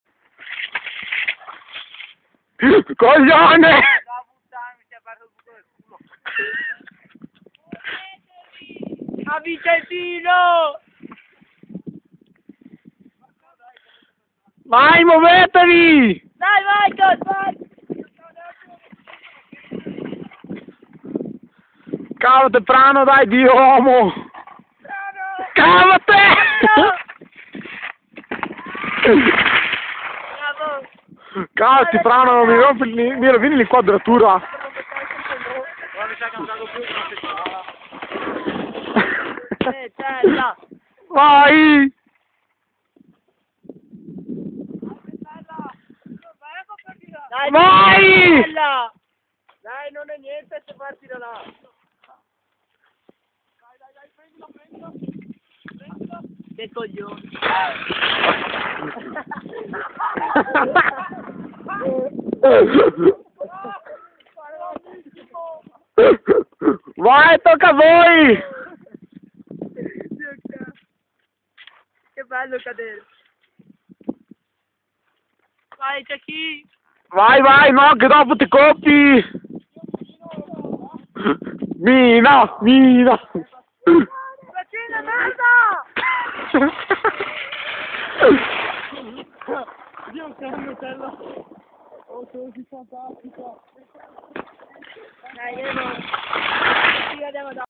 Ski Verena 2000 Belzoni
Ecco gli alunni diligenti dopo la gara di sci. SCANFERLA MICHAEL PRIMOOOOOOOOOOOOOOOOOOO :)